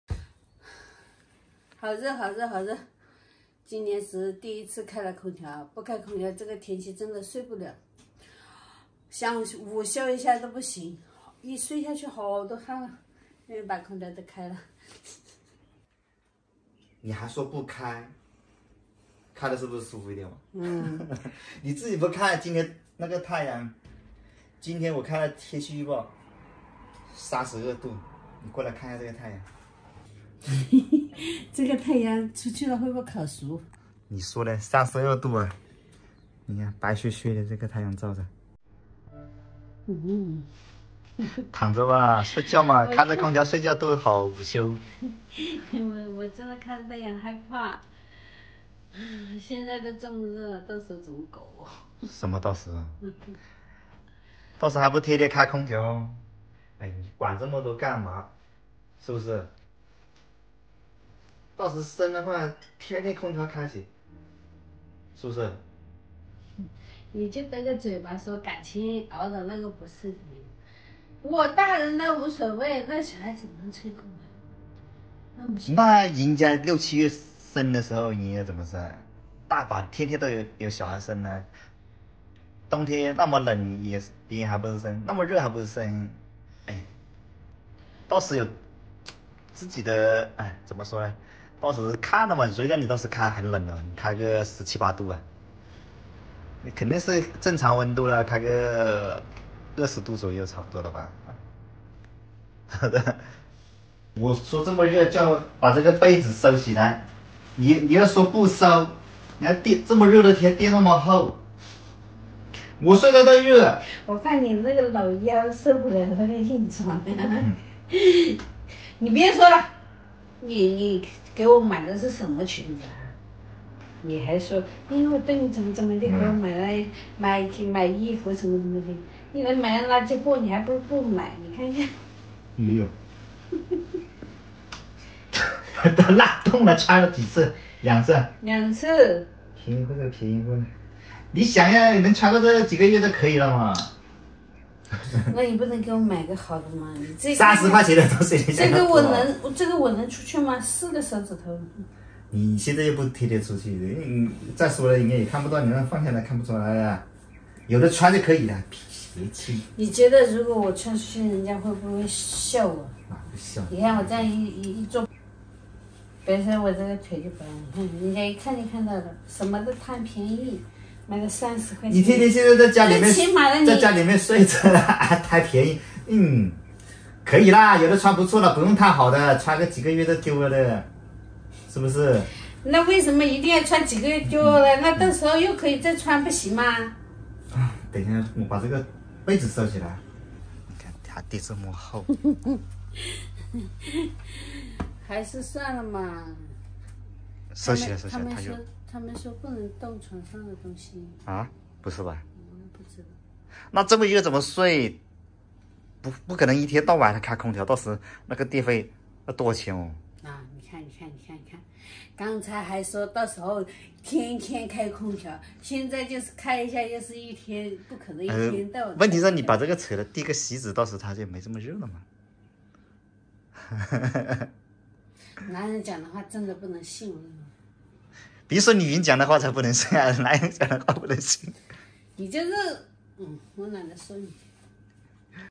小妹第一次开空调，今年天气太热了，到时候坐月子，热得受不了【壮乡小妹】
小妹第一次开空调，今年天气太热了，到时候坐月子，热得受不了
开开心心每天快乐生活一天
记录农村生活日常
record daily life in rural areas
更多【壮乡小妹】视频